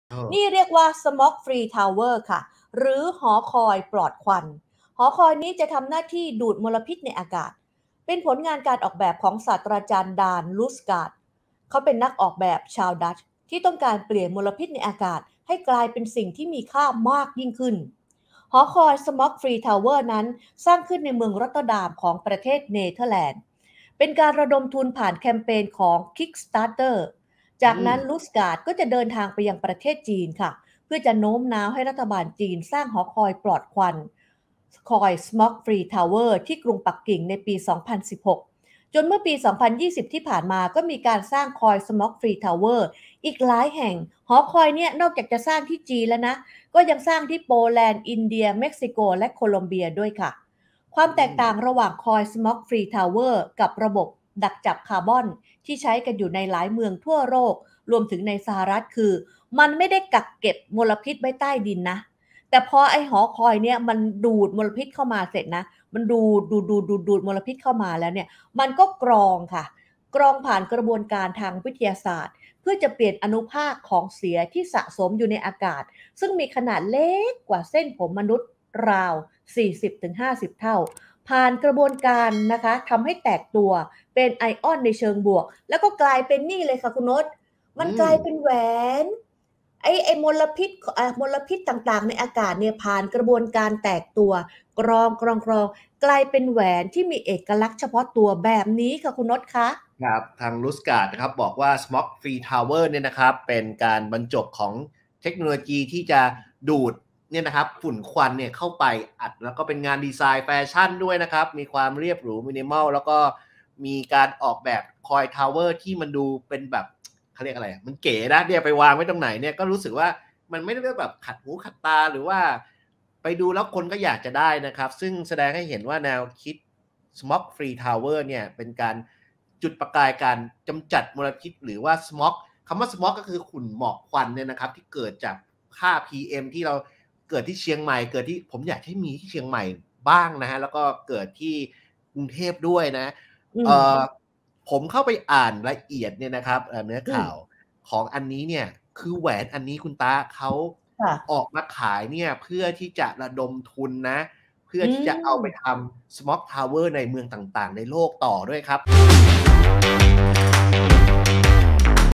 Smog Free Tower หอคอยดูดควัน เปลี่ยนมลพิษให้เป็นแหวนที่มีค่า | beartai BRIEF
Smog Free Tower หรือหอคอยปลอดควัน ทำหน้าที่ดูดมลพิษในอากาศ ซึ่งเป็นผลงานการออกแบบของศาสตราจารย์ Daan Roosegaarde (ดาน รูสการ์ด) นักออกแบบชาวดัตช์ ที่ต้องการเปลี่ยนมลพิษในอากาศให้กลายเป็นสิ่งที่มีค่ามากยิ่งขึ้น

📌 ทันเทรนด์อนาคต กับข่าวสดยามเช้า จันทร์-ศุกร์ 7 โมงเช้า ที่เพจ beartai BRIEF